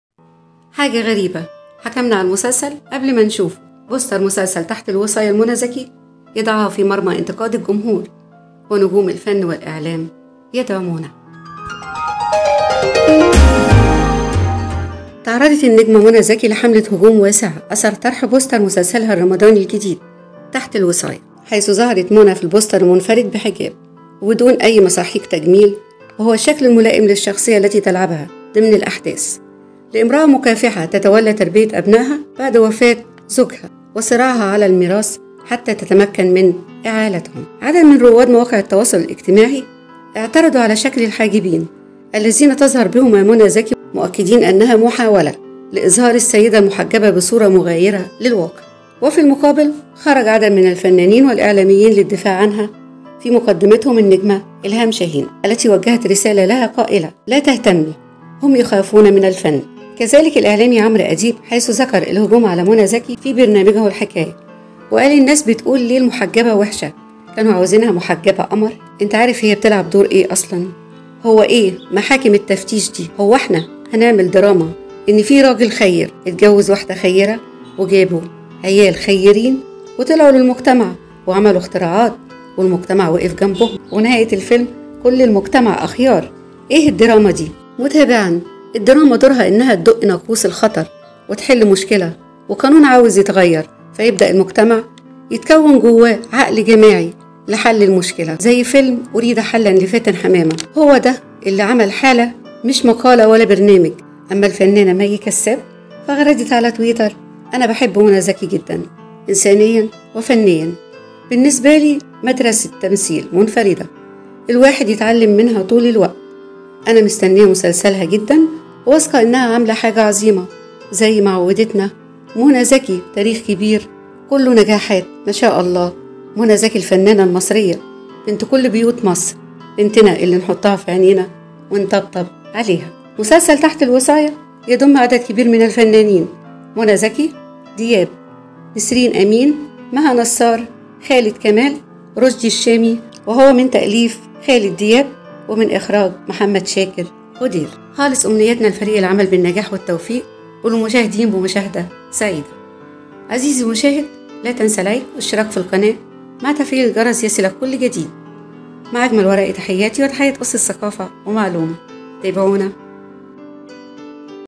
بوستر مسلسل تحت الوصاية ل منى ذكى يضعها فى مرمى انتقاد الجمهور ونجوم الفن والإعلام يدعمونها
بعد ظهور بوستر مسلسل تحت الوصاية وقبل مشاهدة المسلسل قام رواد مواقع التواصل الإجتماعي بانتقاد حاد للنجمة منى ذكى بدون اى وجه حق نقد فى غير محله وقام بعض الفنانين والإعلاميين بدعم منى ذكى شاهد التفاصيل